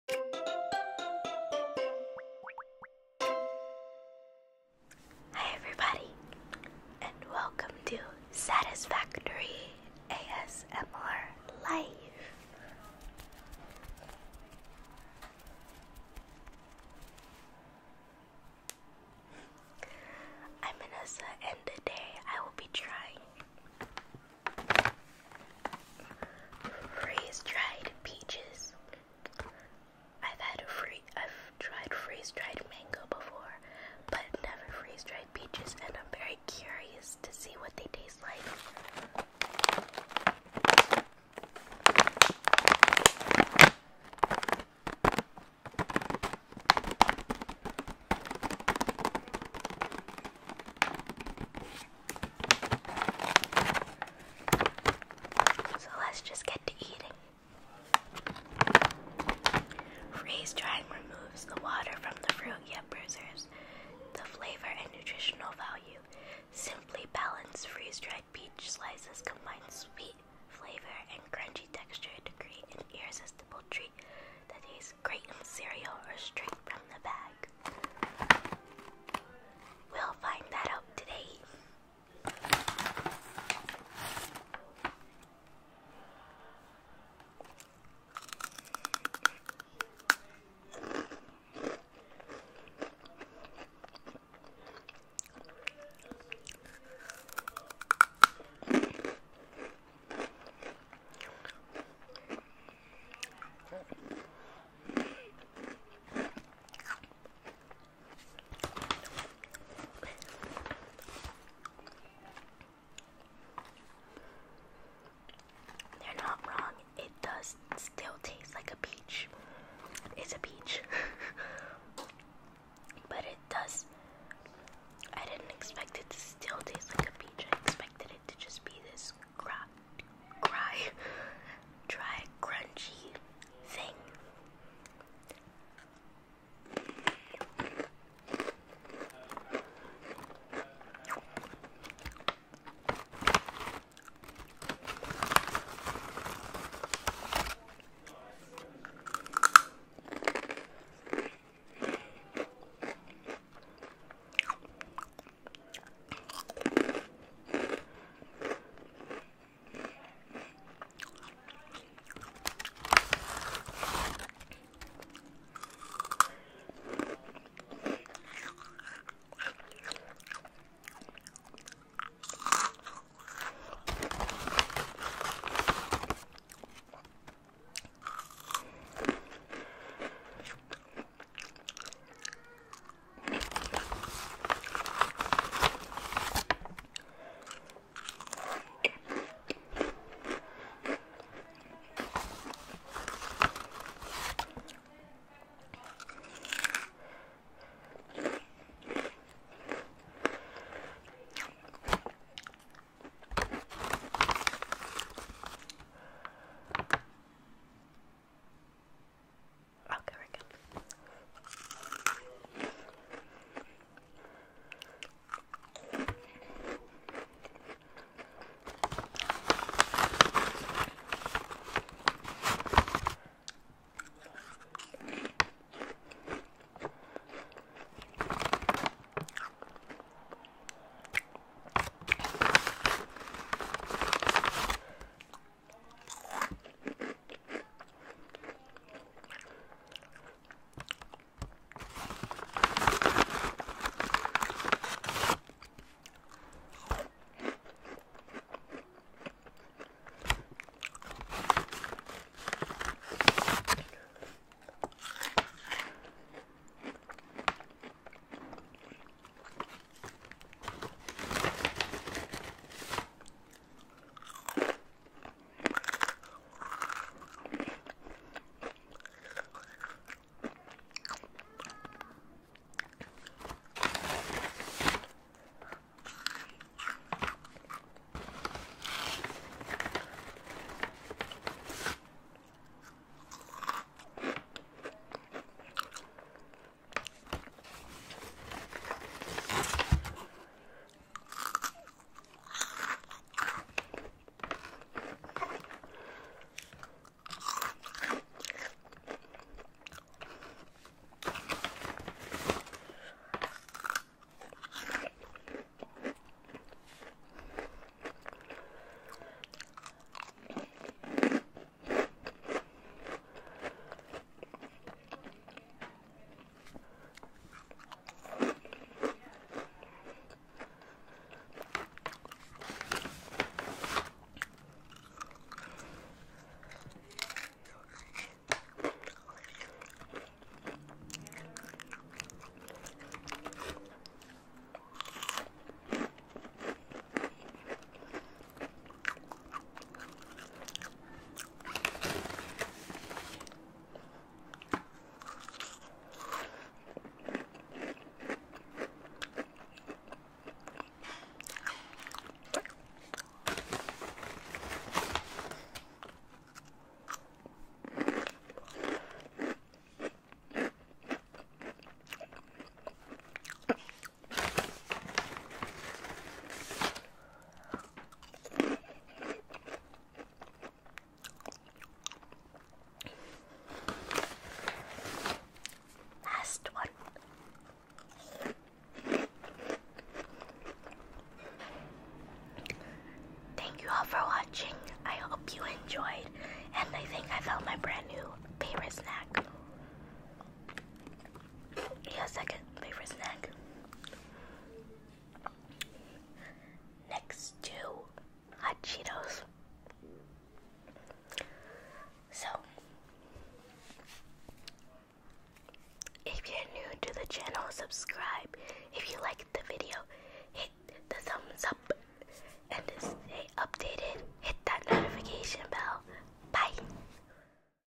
ASMR Eating Freeze Dried Peaches | Crunchy Mouth Sounds
Hi Everyone!
Welcome to my latest foodie adventure. I have been into mouth sounds lately and have to take advantage of all the sweets when I get a chance lol. Enjoy the crunchy mouth sounds of these freeze Dried Peaches.

☆　 FOLLOW ME 　☆

Follow on Twitter @VanessaASMRLife
Follow on IG: SatisfactoryASMRLife